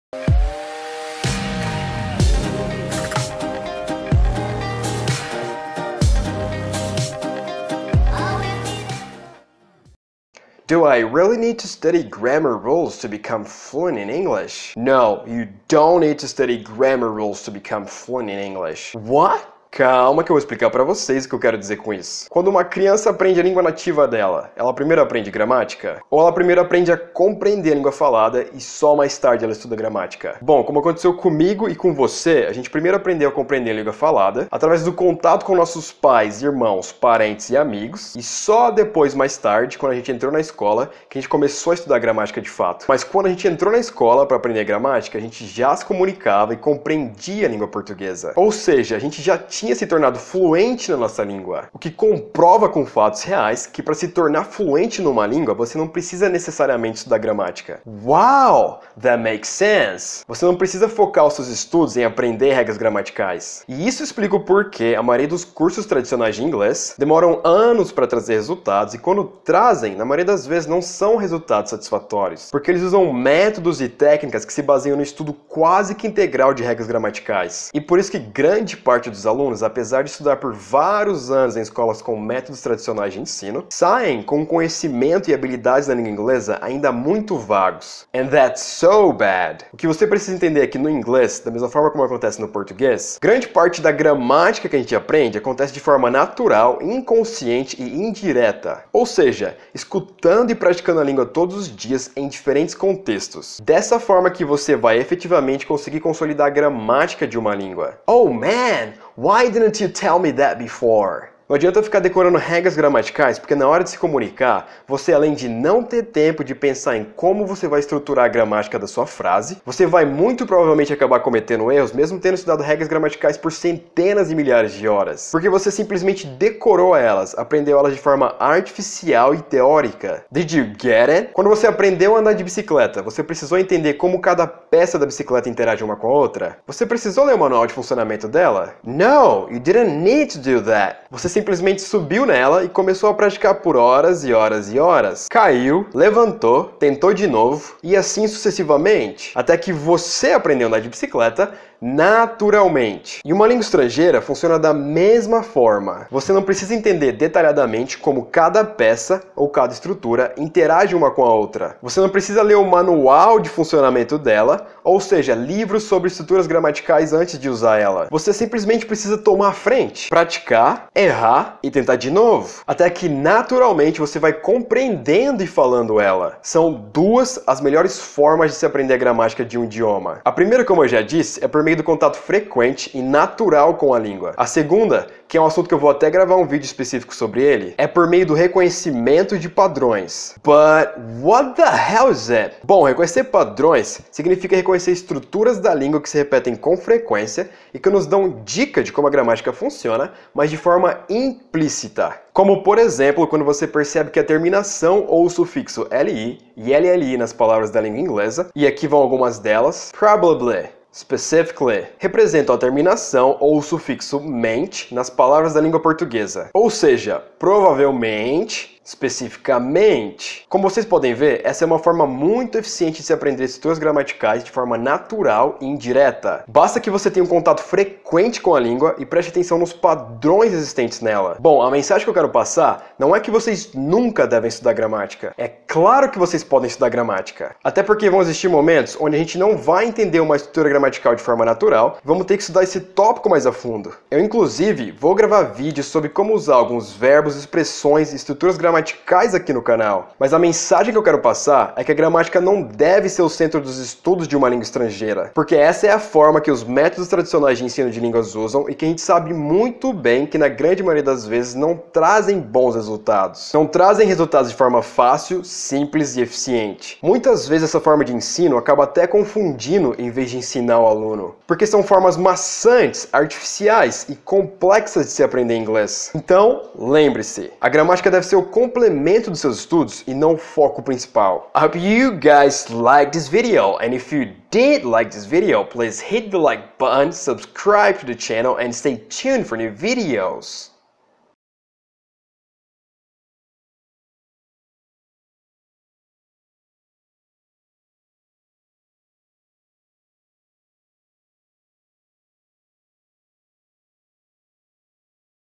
Você NÃO Precisa Estudar GRAMÁTICA! | Aprenda a Aprender Inglês!
Você é uma daquelas pessoas que passou anos estudando gramática Inglesa e simplesmente não entende o porque ainda não consegue se comunicar bem em Inglês? Então esse vídeo é pra você!